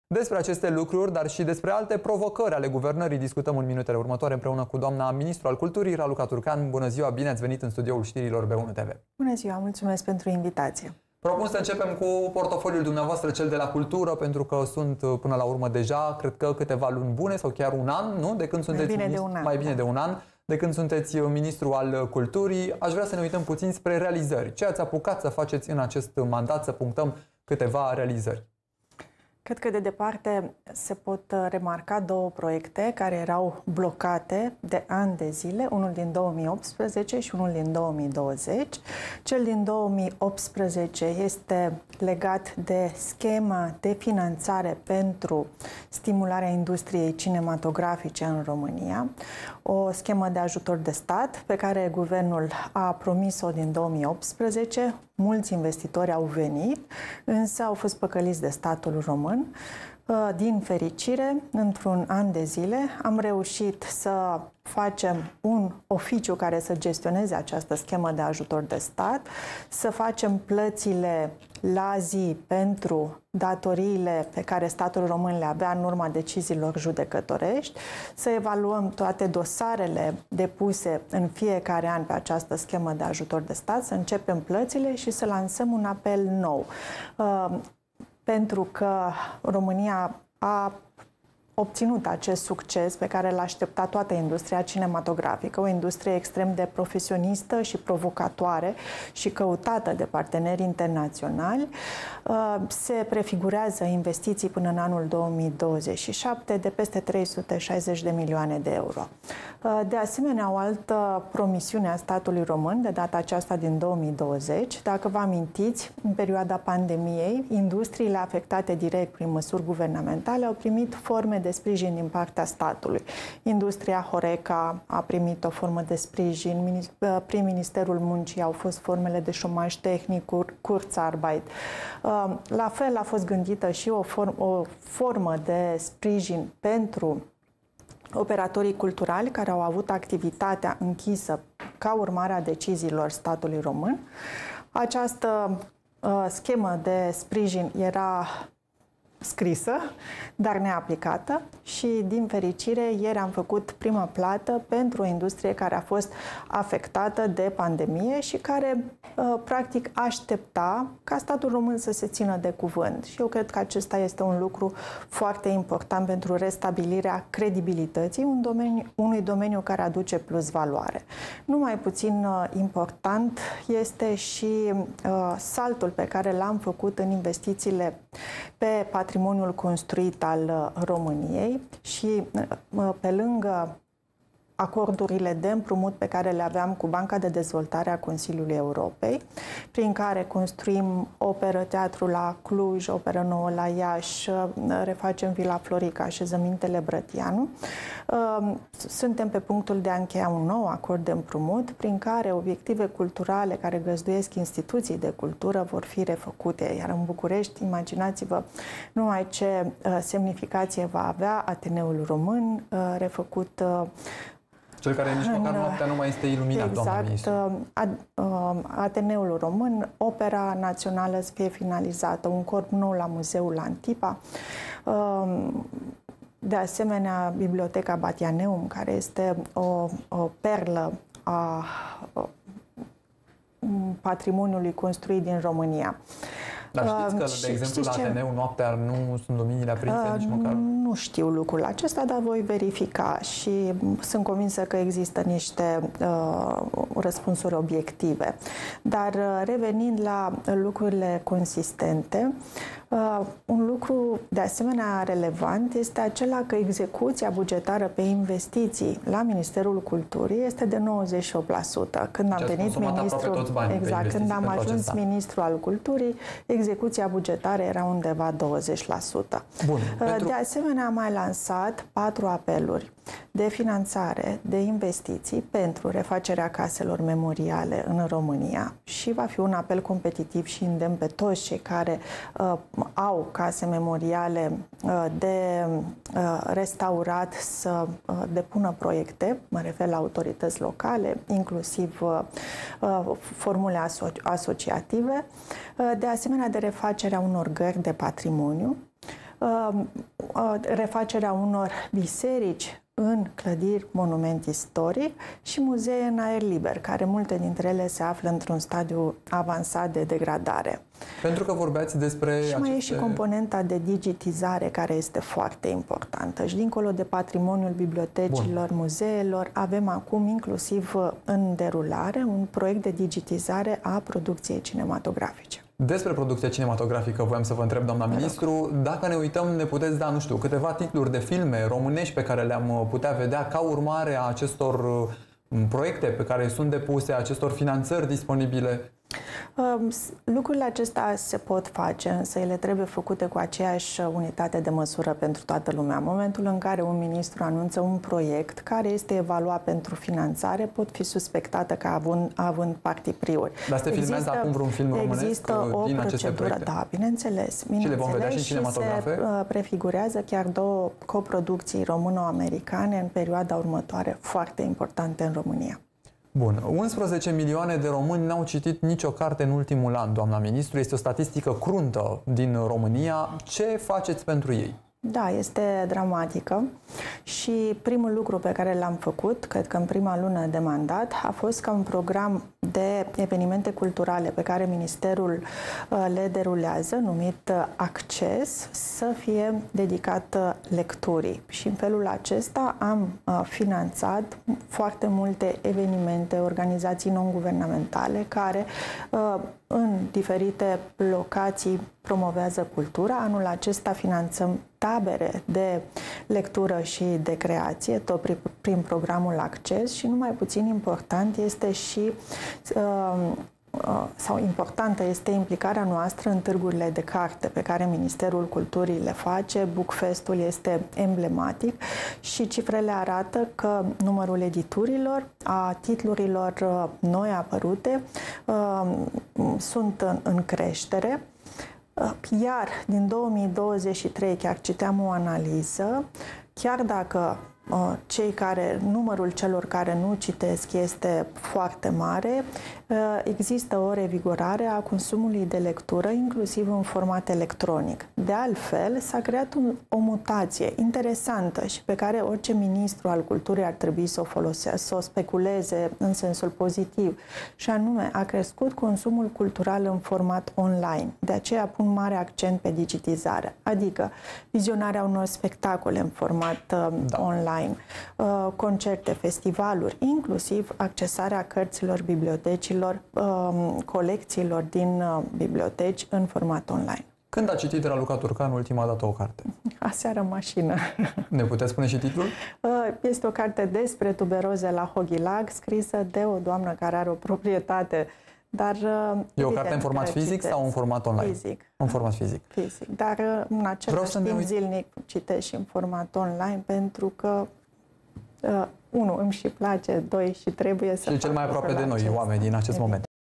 R.Turcan: Există o revigorare a consumului de lectură. A crescut consumul cultural în format online,
o mutație pe care orice ministru al Culturii ar trebui să o speculeze pozitiv.”